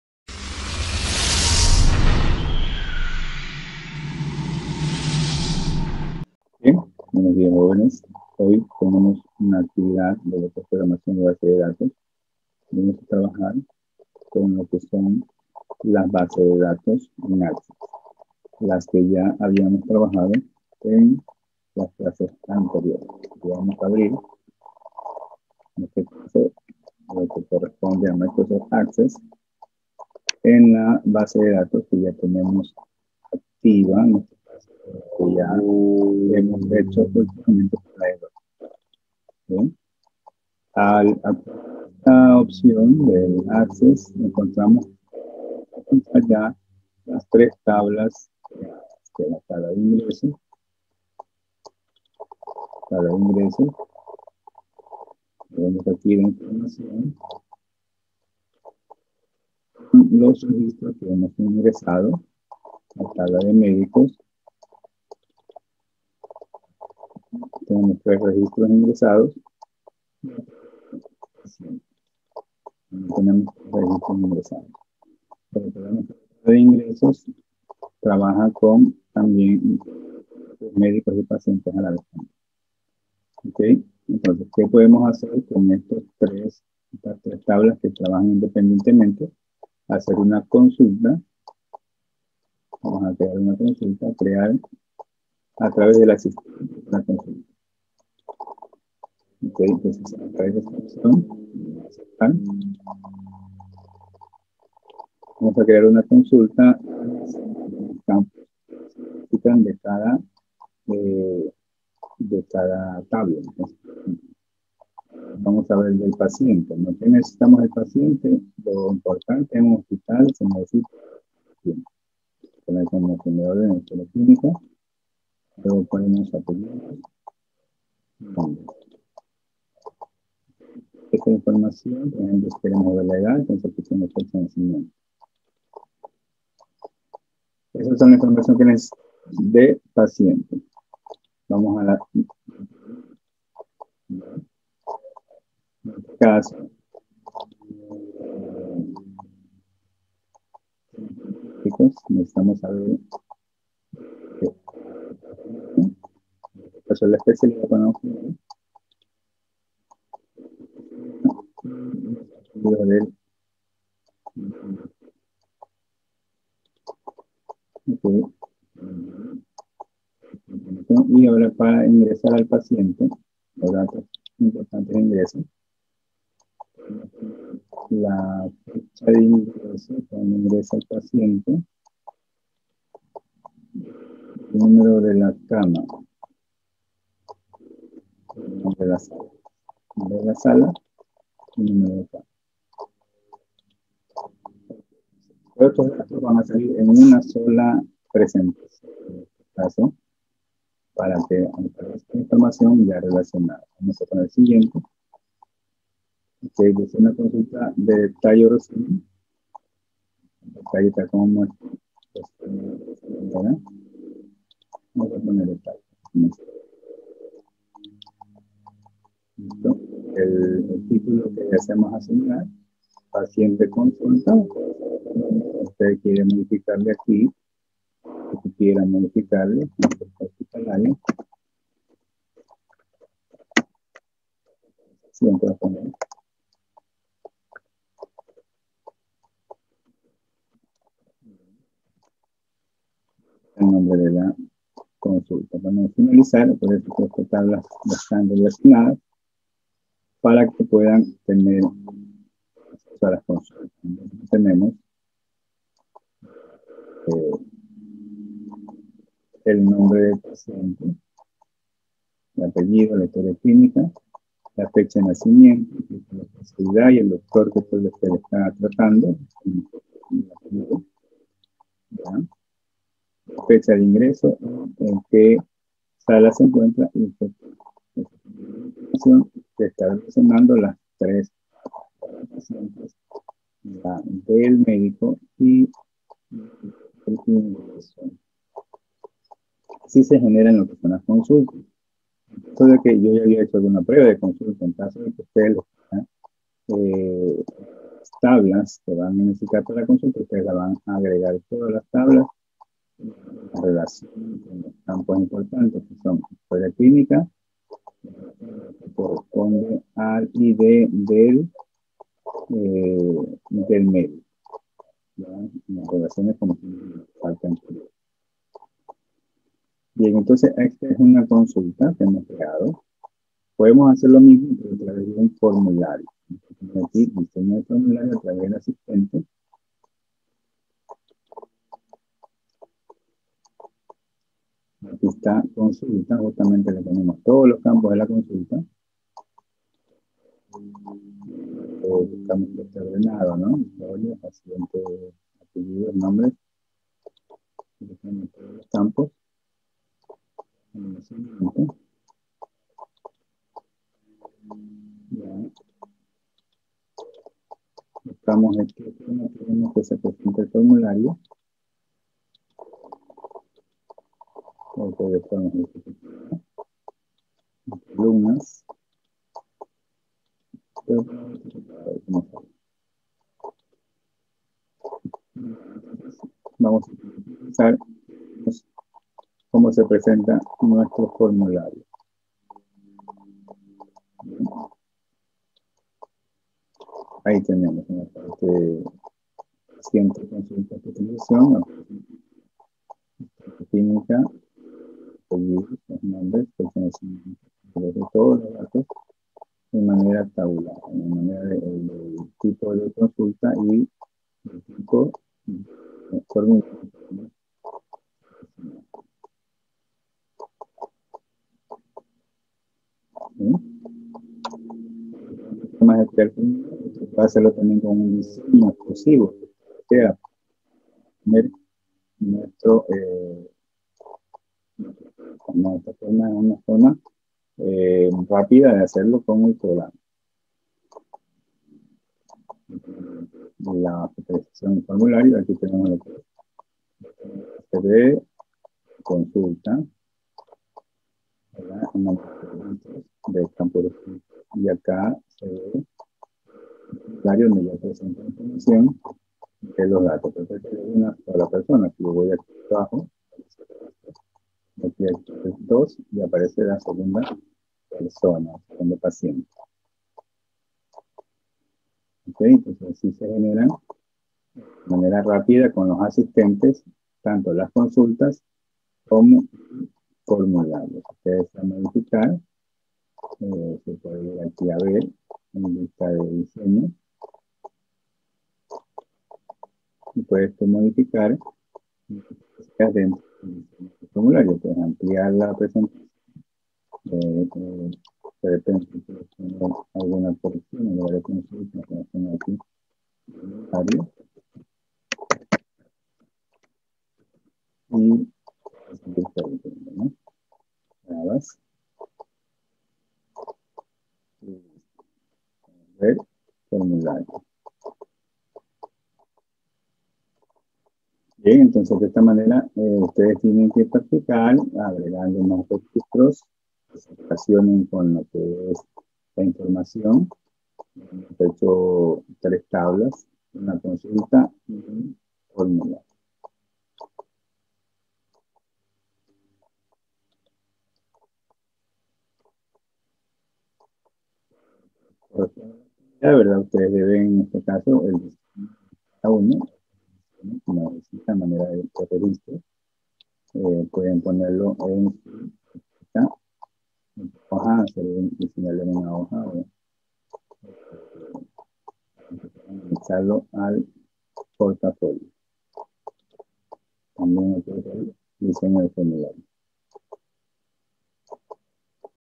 Creación de Base de Datos relacionales en Access: Consultas y Formularios
Clase de 2do Bachillerato perfil Informática Semana 37